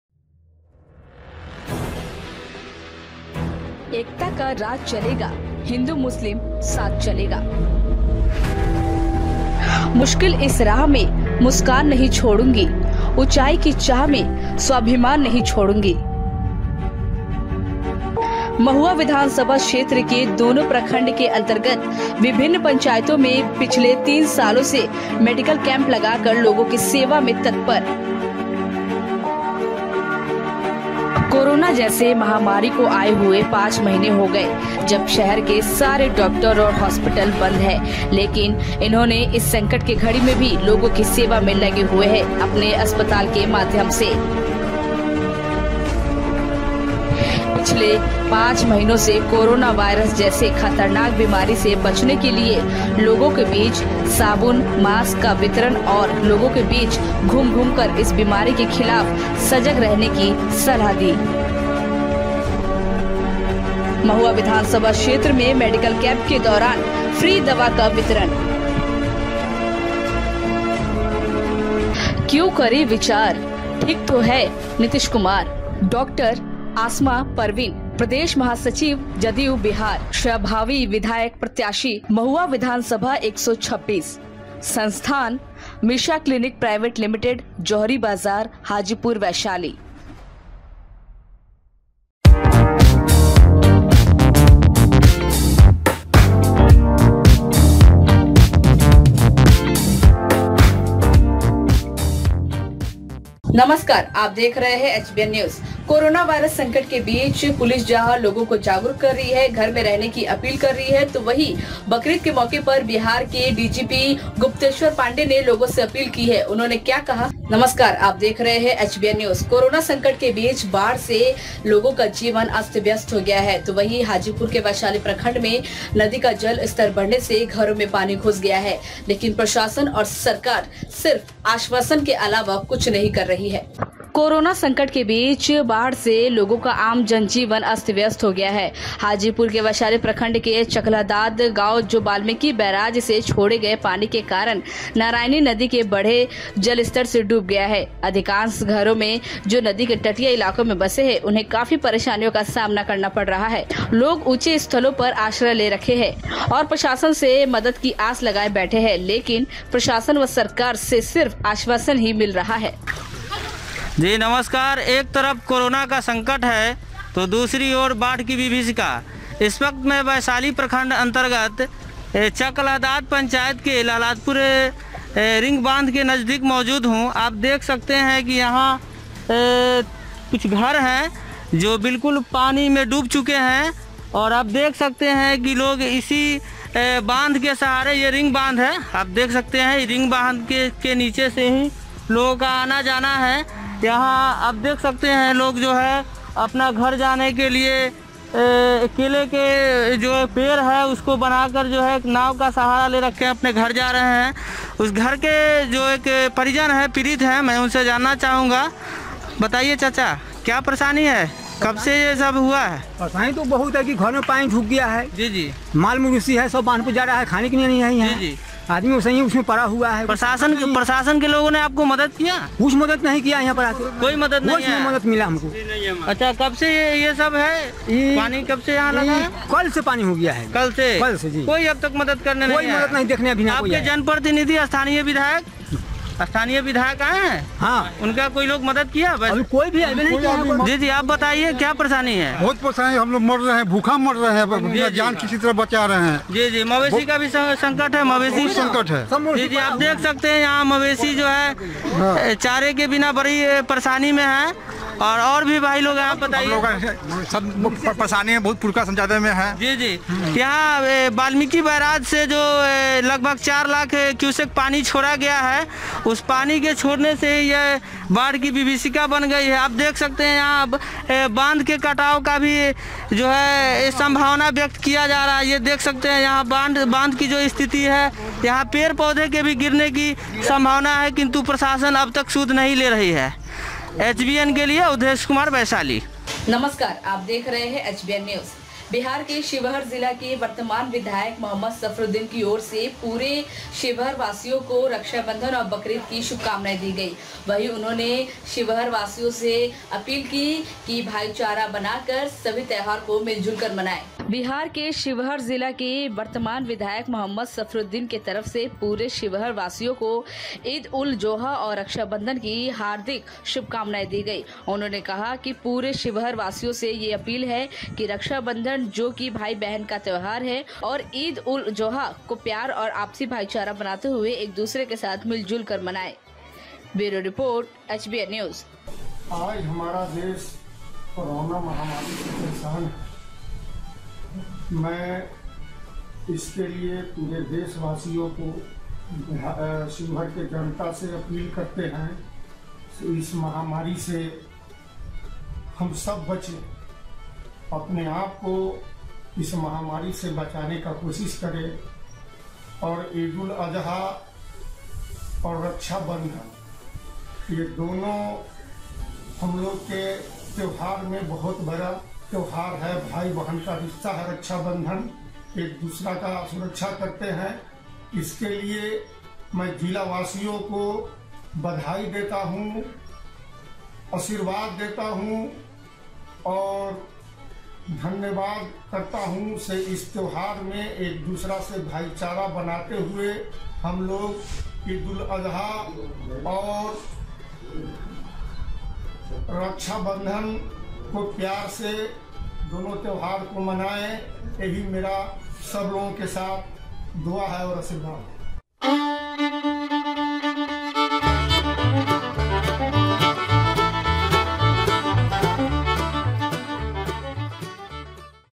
कोरोना संकट में बाढ़ से आम जनजीवन अस्त व्यस्त, नदी के बढ़े जल स्तर से डूबा गांव | HBN News
हाजीपुर संवाददाता अवधेश कुमार की रिपोर्ट
कोरोना संकट में बाढ़ से आम जनजीवन अस्त व्यस्त
       वैशाली प्रखंड के चकअलह दाद गाँव जो बाल्मीकि बैराज से छोड़े गए पानी के कारण नारायणी नदी के बढ़े जल स्तर से डूब गया है।अधिकांश घरों में जो नदी के तटीय इलाकों में बसे हैं उन्हें काफी परेशानियों का सामना करना पर रहा है।लोग ऊंचे स्थलों पर आश्रय ले रखे हैं।तथा प्रशासन से मदद की आश लगाए बैठे हैं किंतु प्रशासन व सरकार से सिर्फ आश्वासन ही मिल रहा है।